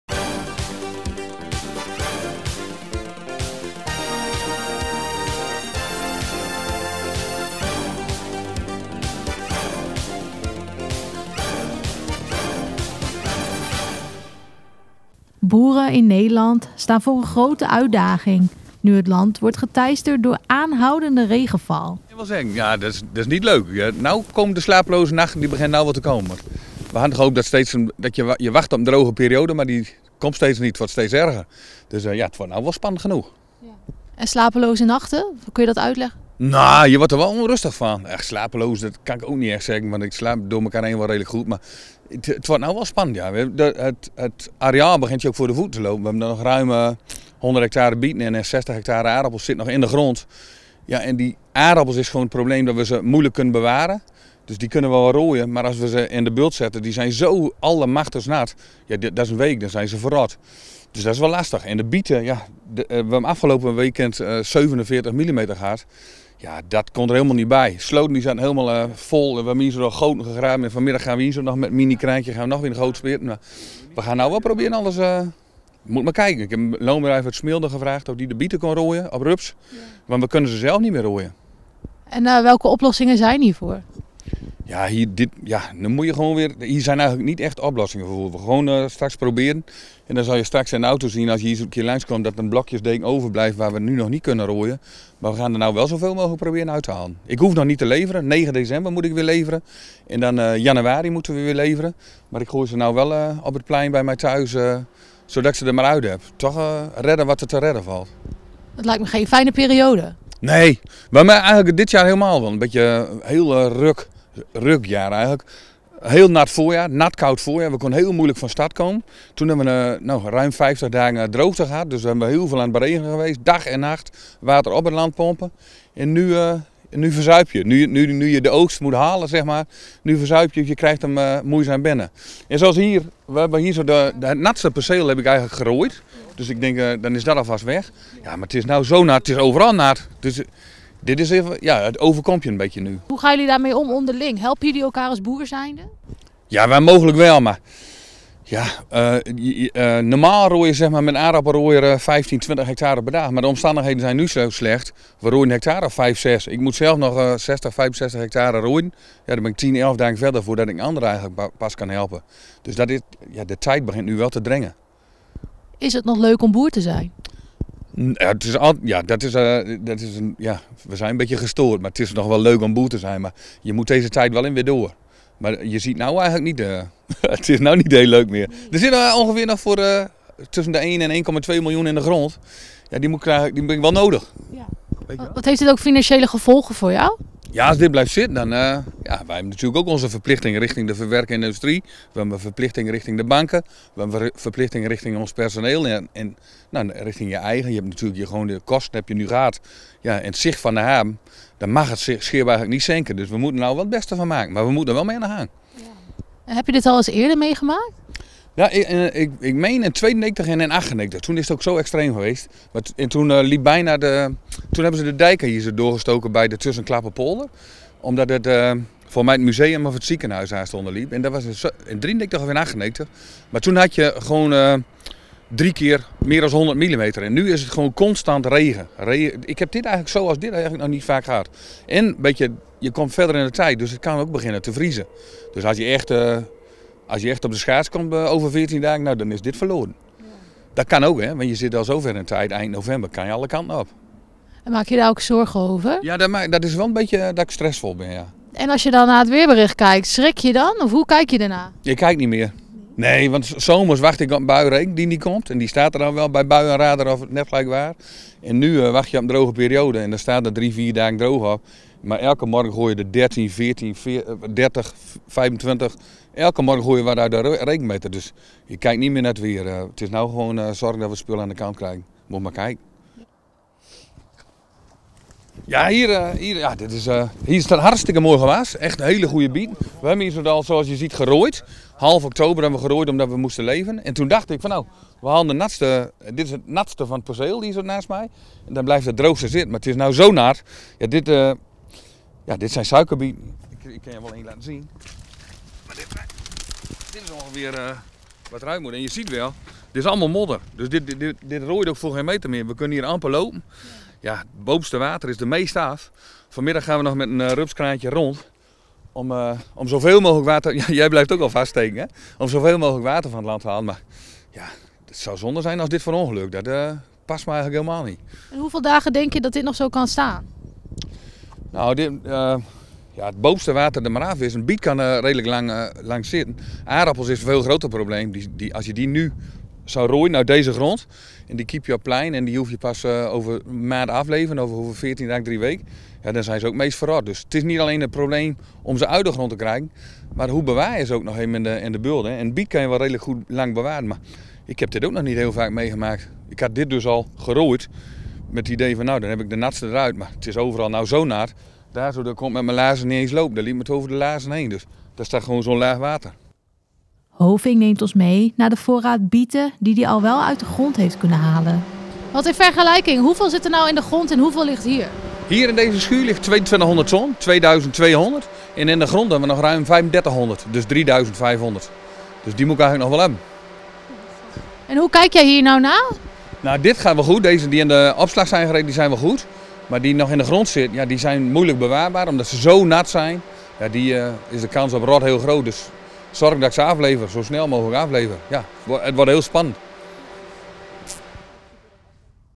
Natte periode teistert boeren
Boeren in Nederland staan voor een uitdaging nu het land wordt geteisterd door aanhoudende regenval. De natte periode, die al weken aanhoudt, heeft geleid tot aanzienlijke moeilijkheden voor agrariërs in het hele land. Van overstromingen tot logistieke problemen, de landbouwsector staat onder druk. Boeren worden geconfronteerd met ondergelopen velden, wat ernstige gevolgen heeft voor gewassen.